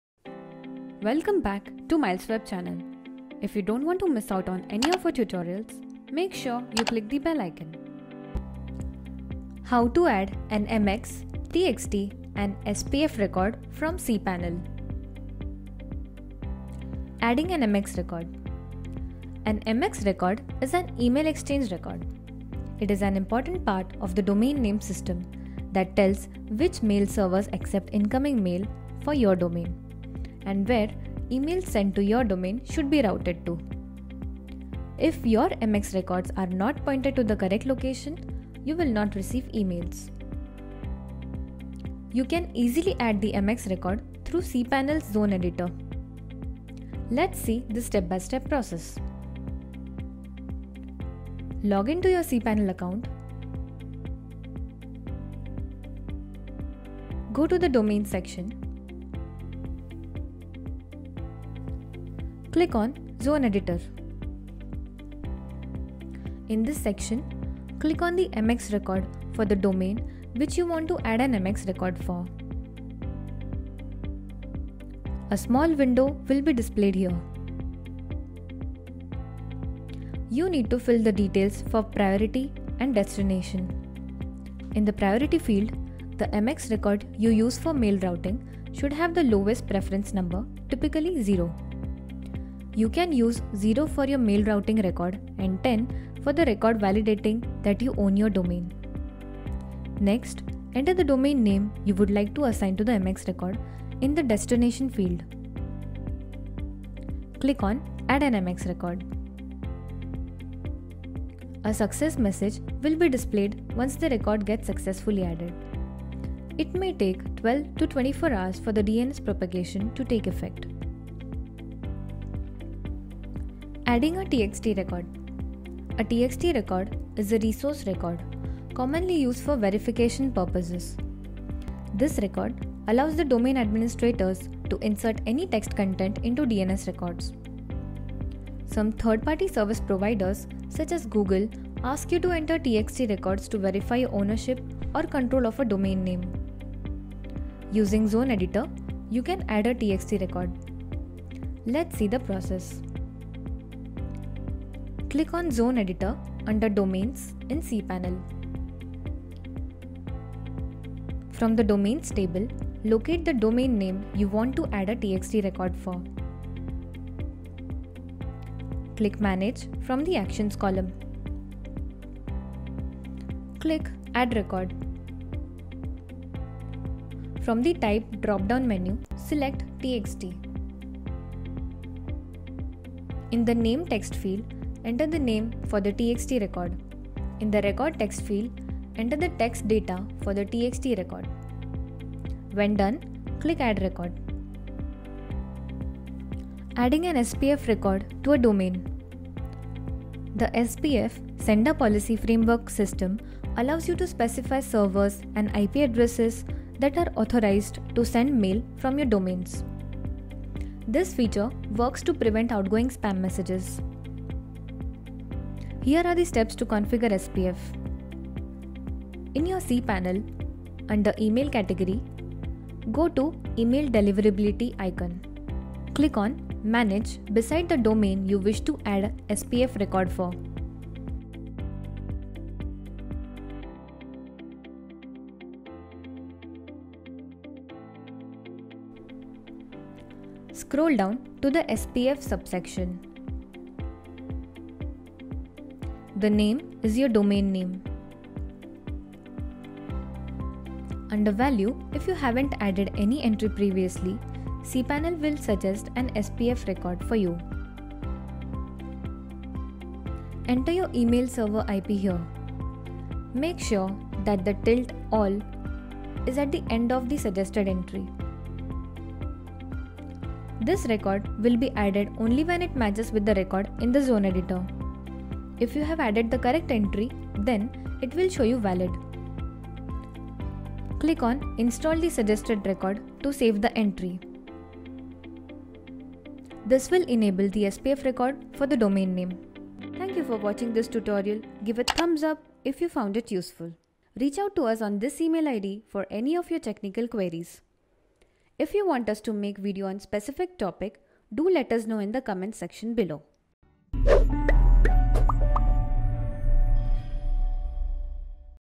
How To Add an MX, TXT & SPF Record from cPanel? | MilesWeb
Learn how to add an MX, TXT & SPF record from cPanel using zone editor.
Contents of this video:
0:00 - Intro
0:16 - How to add MX record
2:03 - How to add TXT record
3:18 - How to add SPF record
Know what is MX, TXT & SPF record:
MX Record
An MX record is an email exchange record. It is an important part of the Domain Name System (DNS) that tells which mail servers accept incoming mail for your domain and where emails sent to your domain should be routed to. If your MX records are not pointed to the correct location, you will not receive an email.
TXT Record
A TXT record is a resource record, commonly used for verification purposes. This record allows domain administrators to insert any text content into DNS records.
For example, some third-party service providers such as Google, ask you to enter TXT Records to verify your ownership or control of a domain name.
SPF Record
The SPF (Sender Policy Framework) system allows you to specify servers and IP addresses that are authorized to send mail from your domain(s). This feature works to prevent outgoing spam messages.